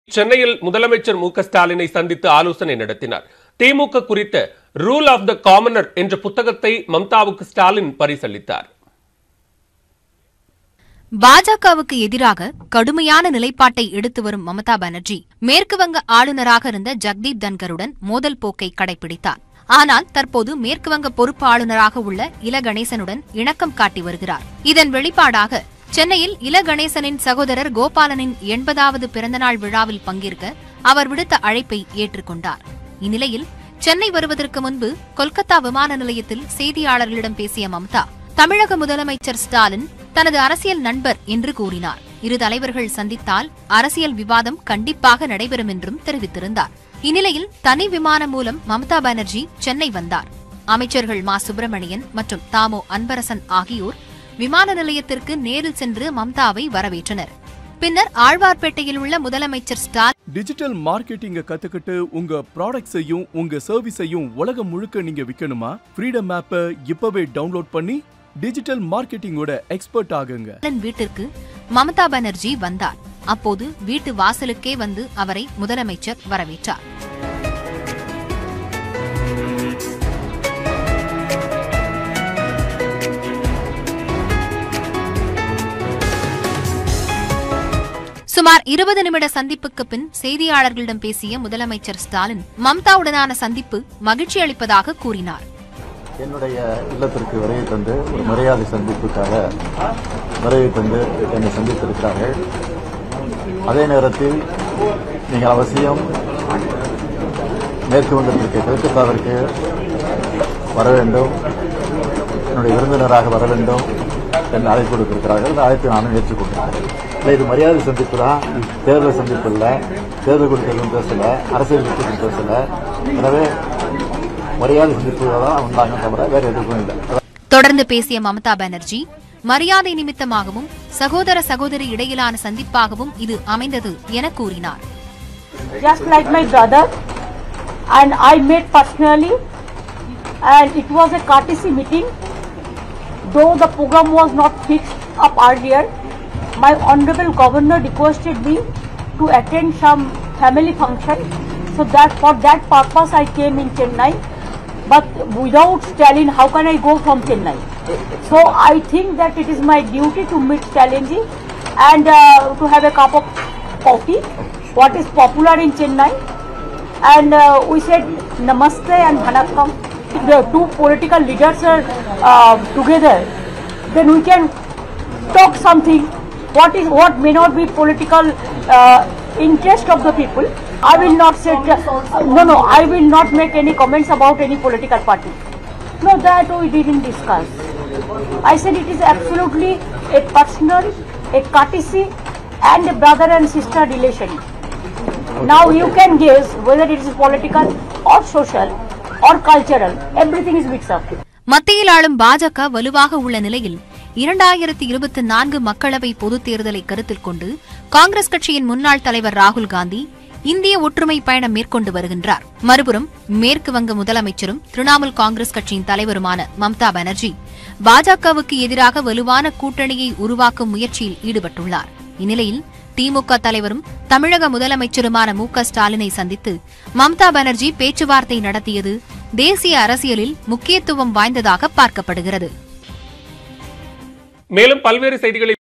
West Bengal CM Mamata Banerjee Meets TN CM MK Stalin | முதல்வர் முக ஸ்டாலினை சந்தித்த மம்தா பானர்ஜி
West Bengal CM Mamata Banerjee Meets TN CM MK Stalin | முதல்வர் முக ஸ்டாலினை சந்தித்த மம்தா பானர்ஜி  | செய்திகளை துல்லியமாகவும் துரிதமாகவும் வழங்கும் தமிழகத்தின் முதன்மை செய்தி தொலைக்காட்சி உங்கள் நியூஸ் 18 தமிழ்நாடு